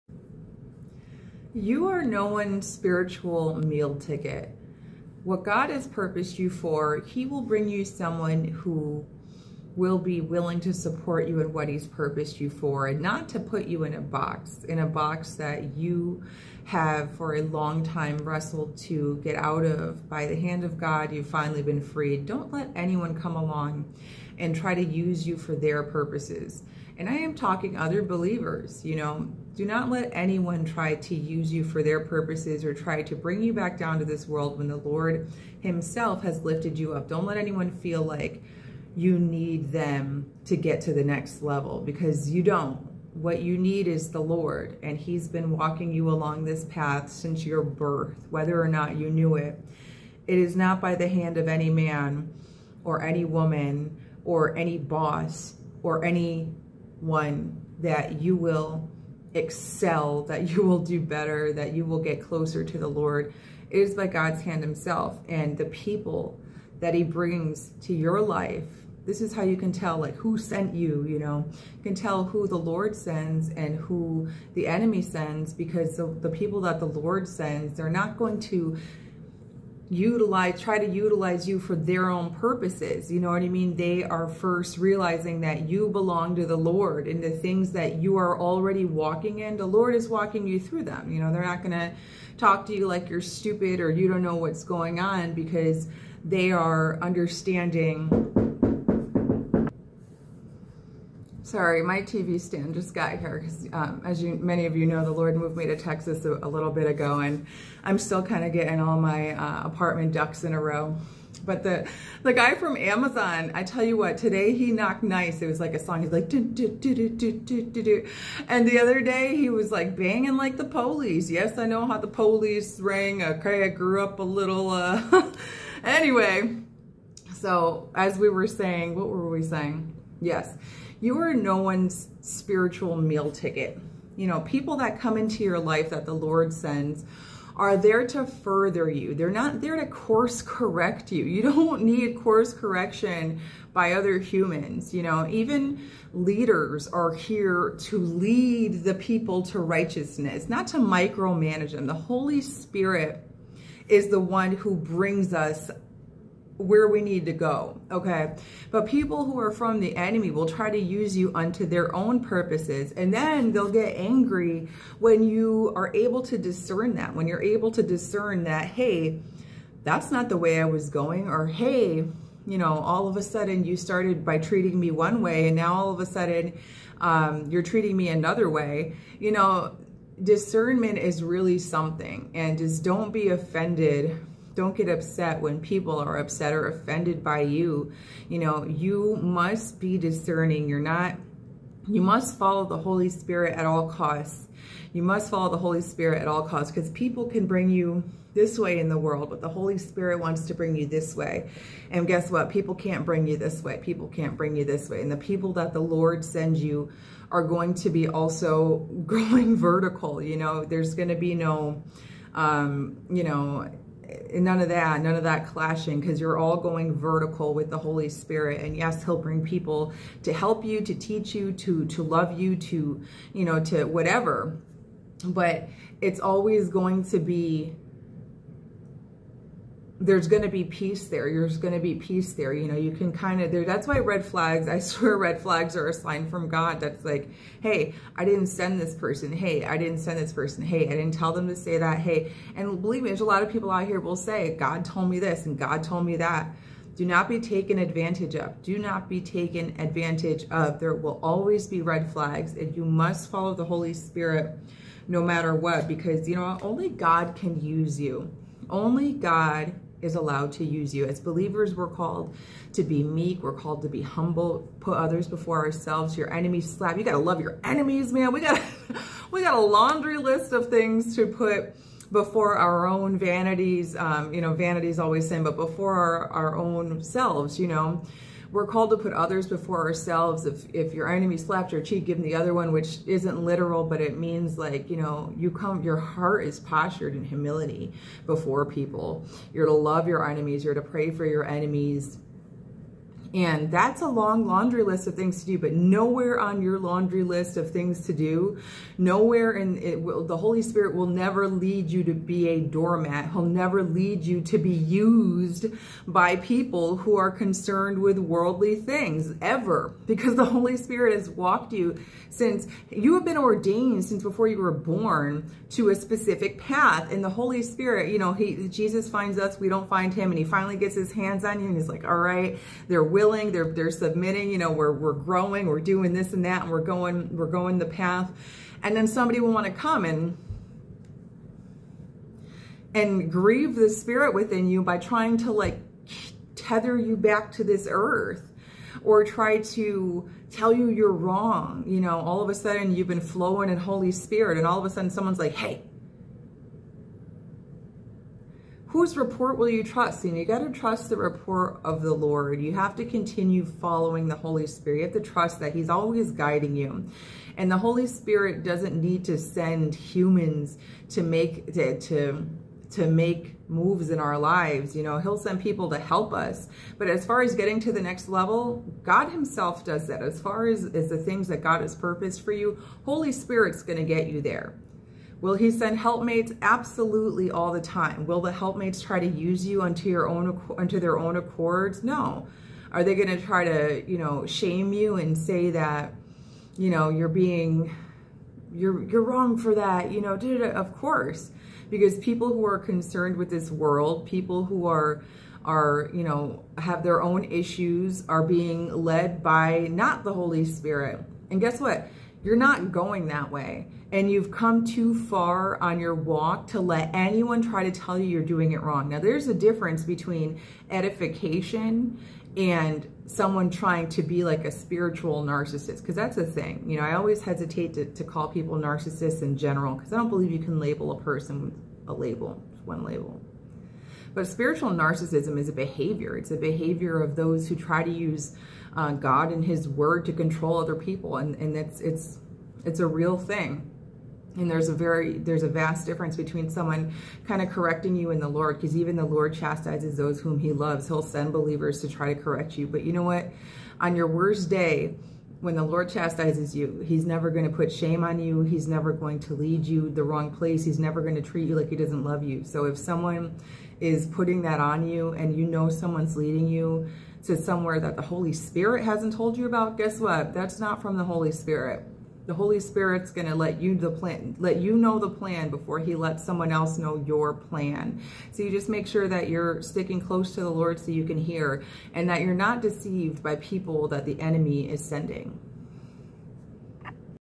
You're No One's Spiritual Meal Ticket *Spiritual Narcissism* 🎟
2 Timothy 3:4-5 traitors, headstrong, haughty, lovers of pleasure rather than lovers of God, 5 having a form of godliness but denying its power. And from such people turn away!

1 Corinthians 5:11 But I now have written unto you not to keep company with any man who is called a brother if he is a fornicator, or covetous, or an idolater, or a railer, or a drunkard, or an extortioner. With such a one you are not even to eat.

Peace All 🕊🤍🪽